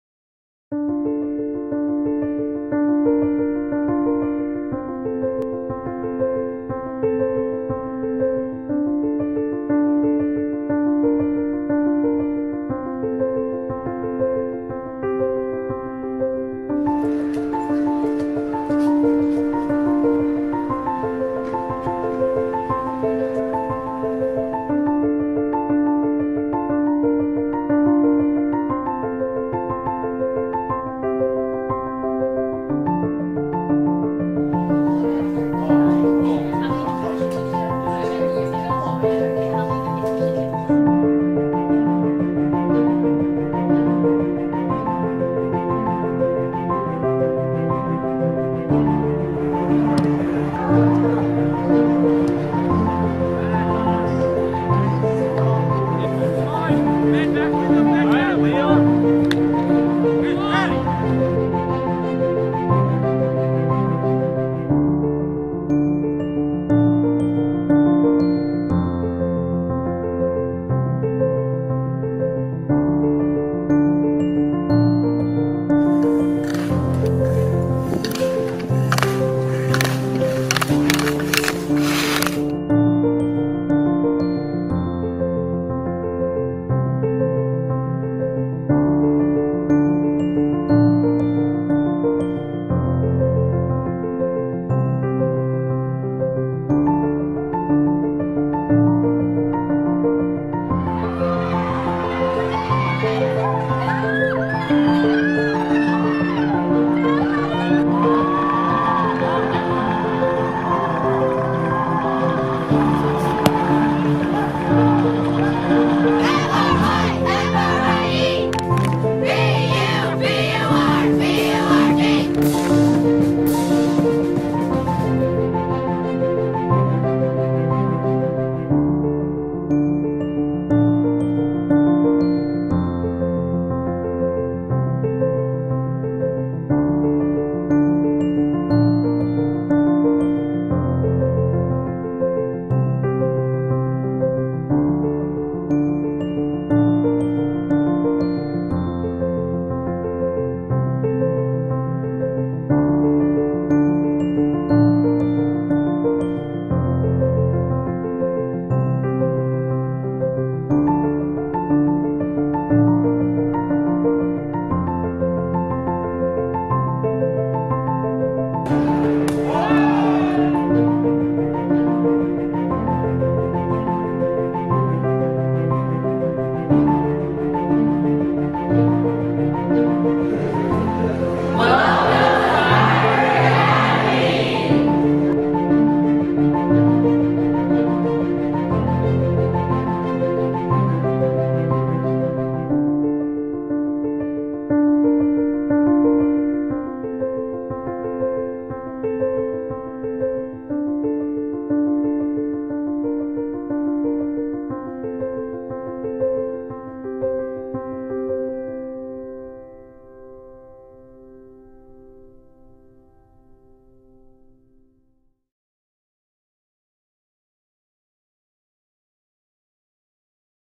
Fryeburg Academy moments
Fryeburg Academy thành lập năm 1792, là trường PTTH tư thục nội trú lâu đời và danh tiếng tại bang Maine, Đông Bắc Mỹ.